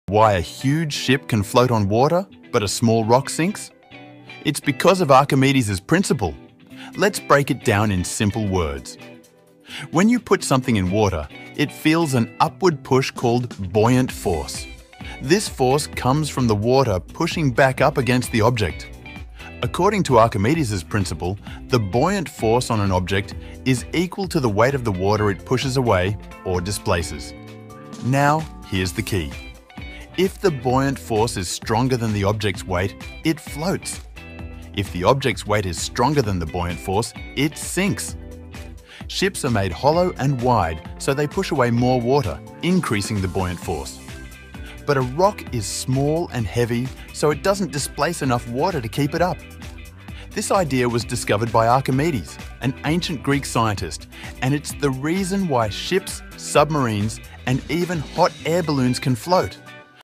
Why Do Boats Float and Rocks Sink? 🚢⚖️ Archimedes' what floats and sinks in water,sink and float
Why do huge ships float while small rocks sink? 🚢⚖️ It all comes down to Archimedes' Principle! This fundamental law of physics explains buoyant force—the upward push water gives to objects. If an object pushes away (or displaces) enough water to create a buoyant force greater than its weight, it floats*l. If not, it sinks!

Ships are designed to be hollow and wide, allowing them to displace more water and stay afloat, while dense objects like rocks sink instantly. This principle, discovered by Archimedes, is used in ships, submarines, and even hot air balloons!

🌊 Watch now to understand this amazing science in simple words!